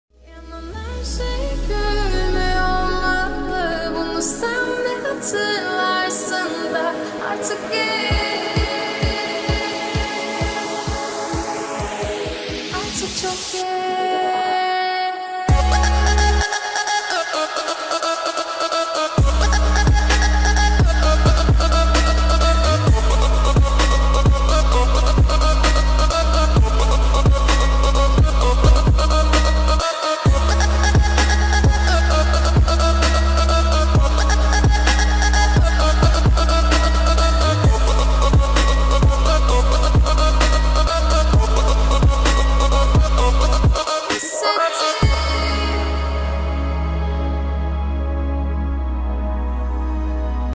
Lunar Rabbit Mortar Piece - short version
Uzun uzun anlatımlar dinlemek istemeyenlere, Lunar Rabbit Mortar Piece nasıl yapılır hızlı gösterim hizmetinizde!